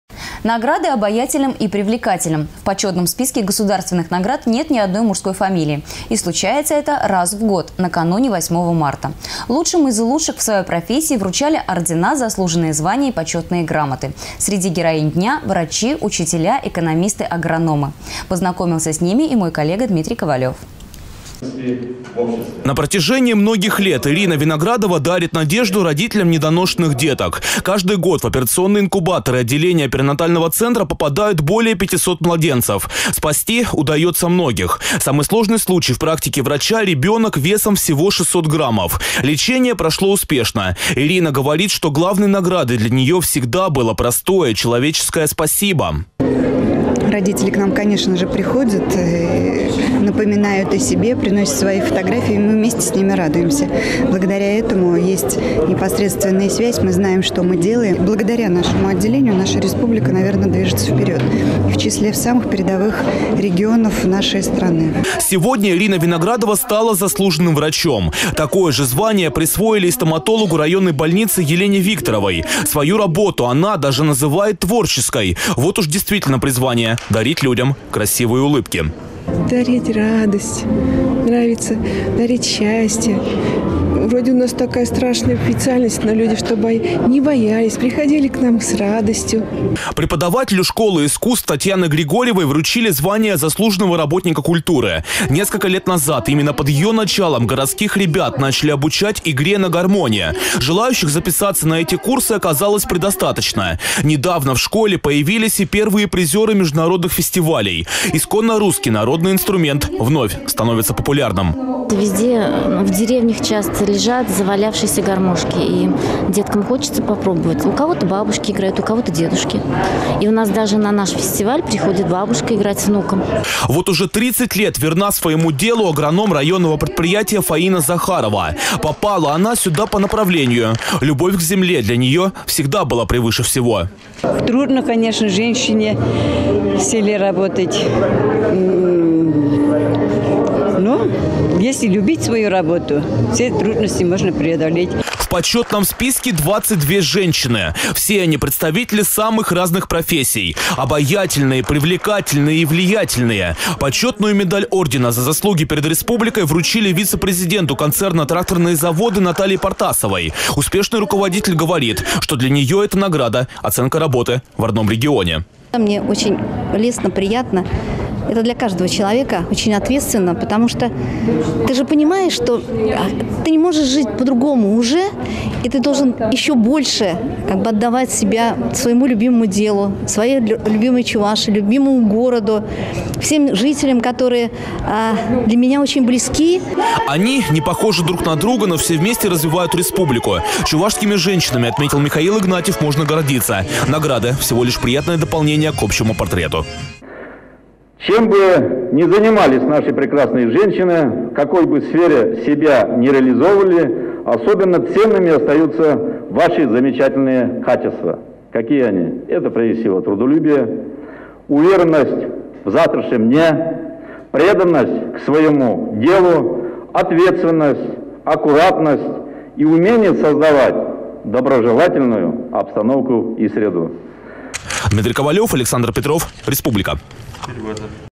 Награды обаятельным и привлекательным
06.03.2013
Раз в году, только накануне восьмого марта, в доме правительства поощряют самых успешных женщин. В этом почётном списке нет ни одной мужской фамилии. Виновницы торжества - представительницы прекрасного пола. Лучшим из лучших в своей профессии вручали ордена, заслуженные звания и почётные грамоты. Среди героинь дня врачи, учителя, экономисты, агрономы.